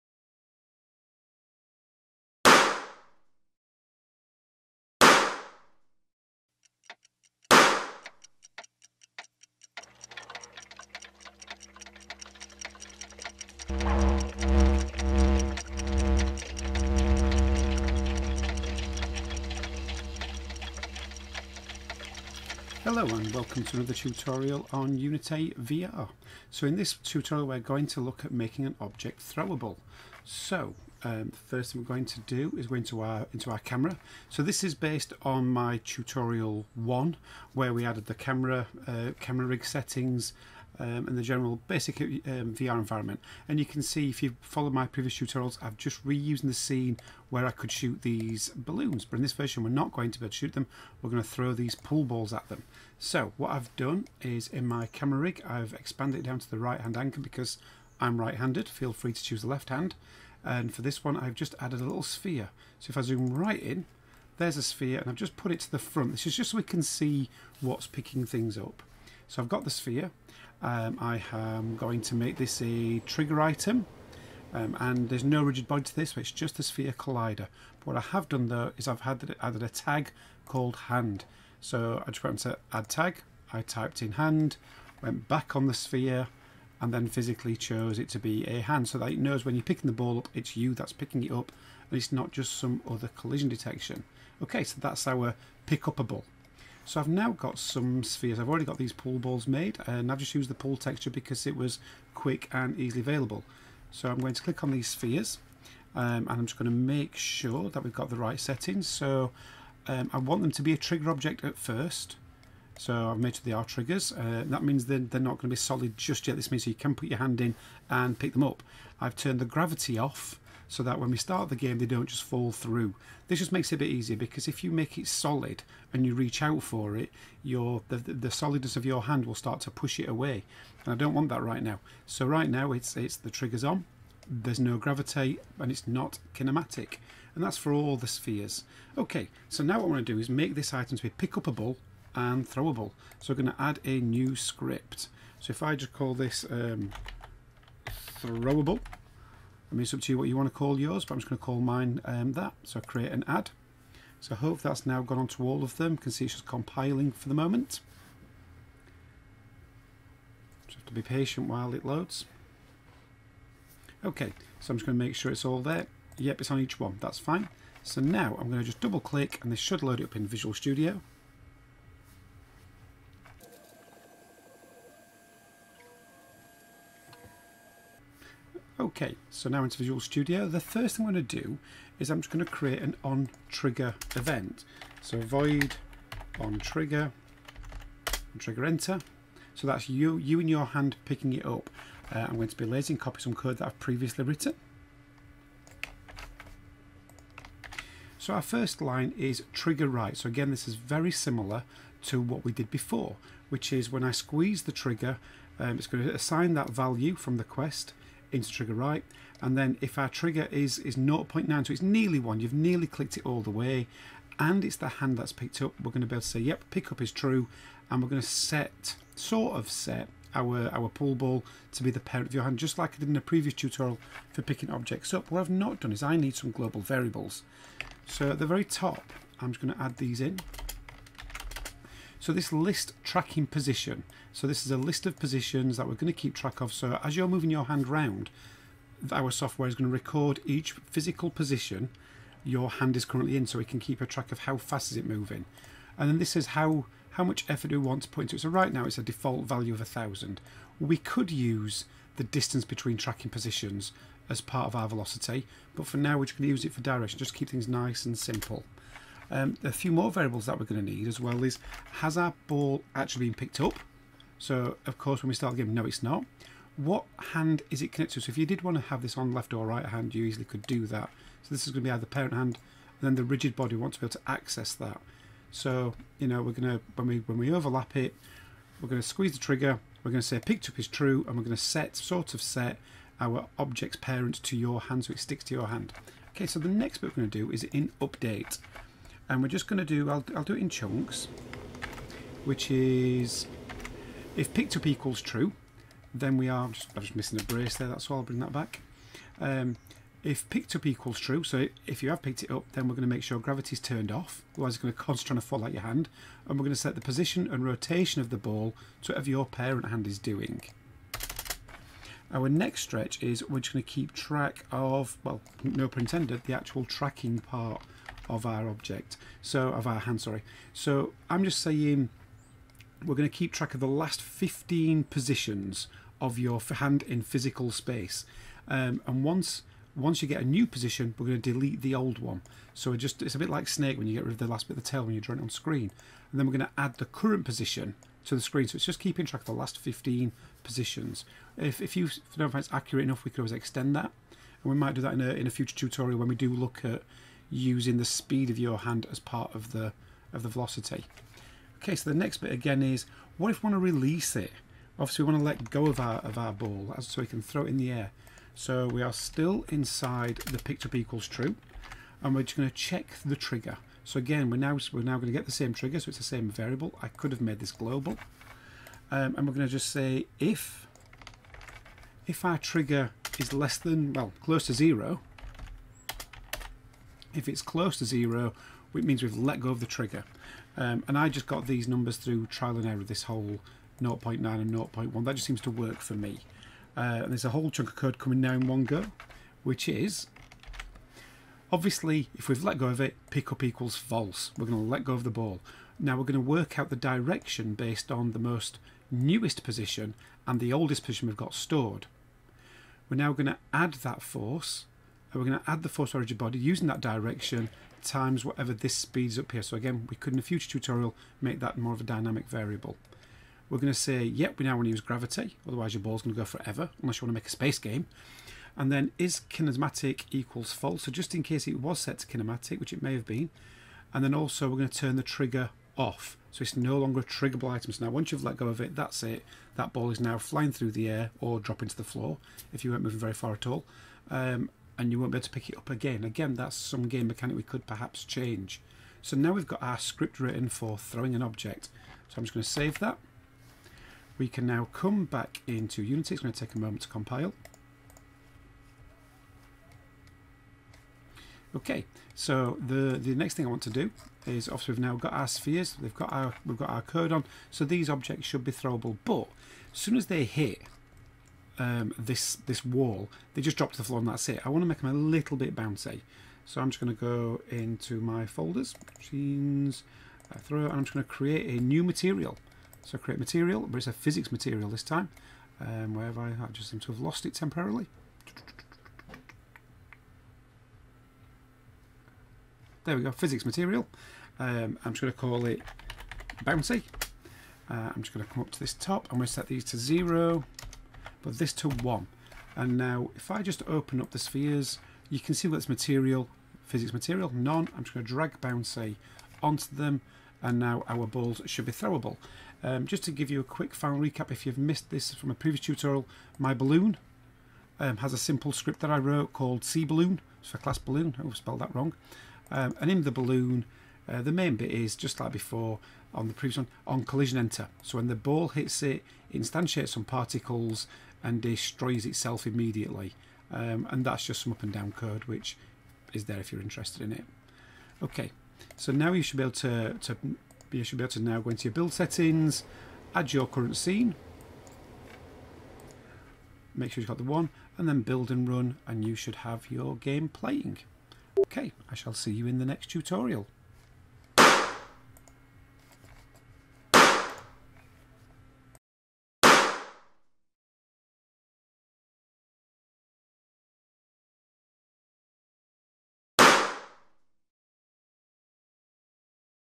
Throwing Objects in VR using Unity
Throwing an object in VR using Unity
Tested using a quest, but should work with most VR Environments.